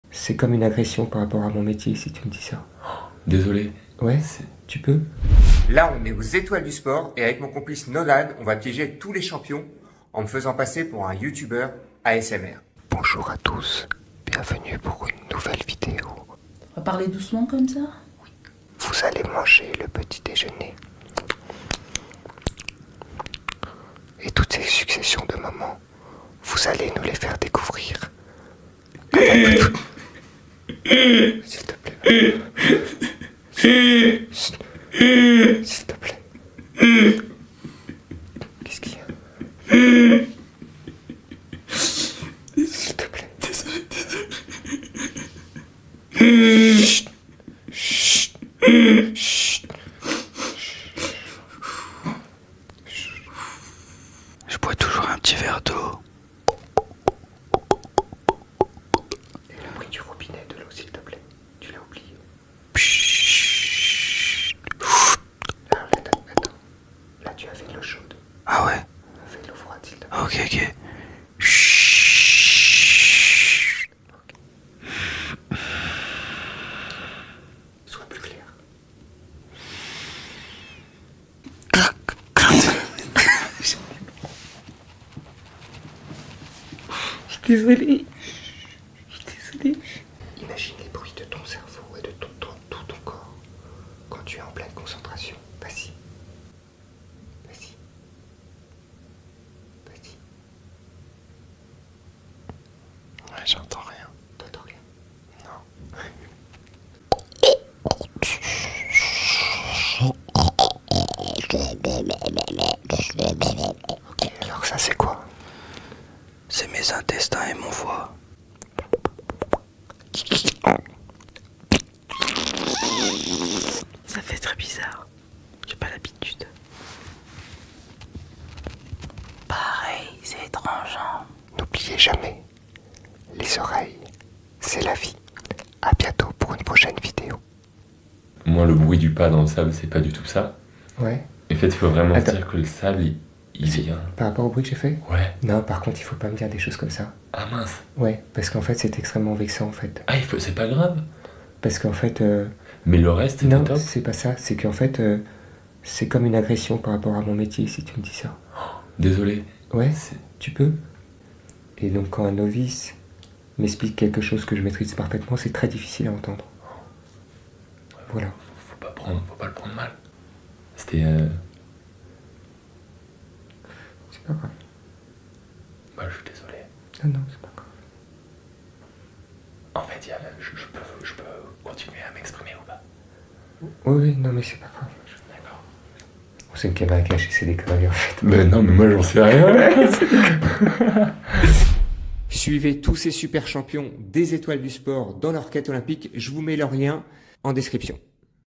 PRANK: CHOQUÉS par un Youtubeur ASMR
Je me fais passer pour un youtubeur ASMR.
Comment les grands champions des étoiles du sport vont-ils réagir ?